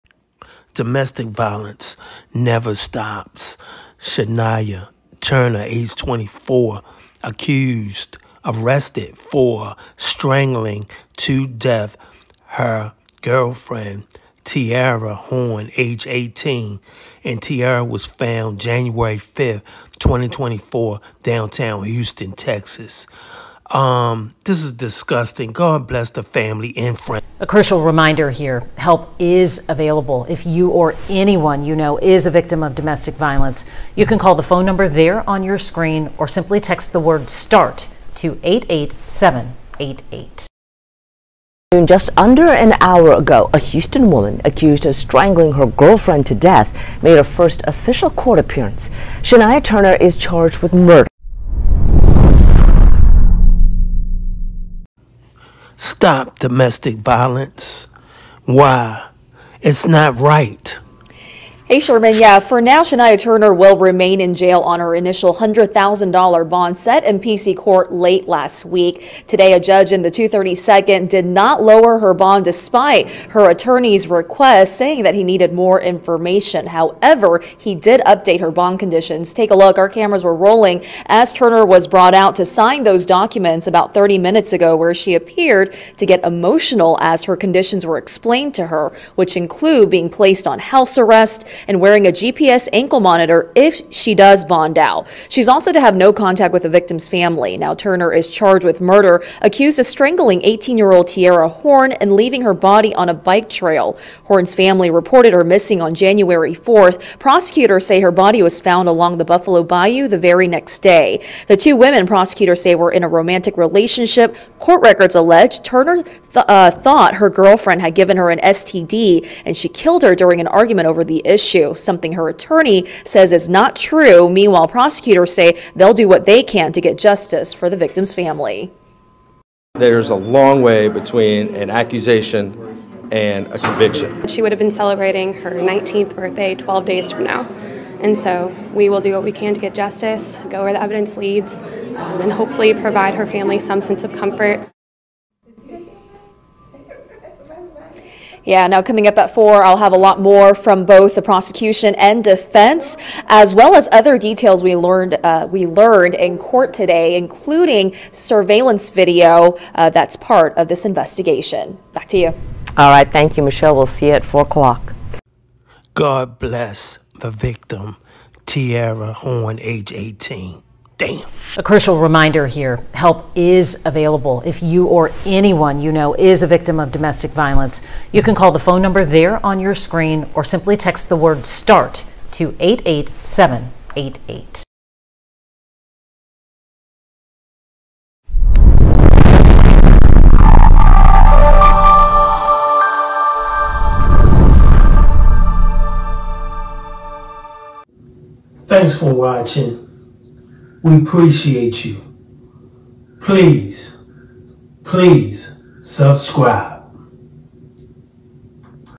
TIERRA TURNER 18 WAS MISSING FOUND DEAD! HER GF SHANIA L. TURNER 24 ARRESTED JAN 5, 2024 HOUSTON, TX
DOMESTIC!!! TIERRA TURNER 18 WAS MISSING FOUND DEAD! HER GF SHANIA L. TURNER 24 ARRESTED JAN 5, 2024 HOUSTON, TX

YOUTUBE.COM/@REALWOMENMURDERSNOTFICTION9264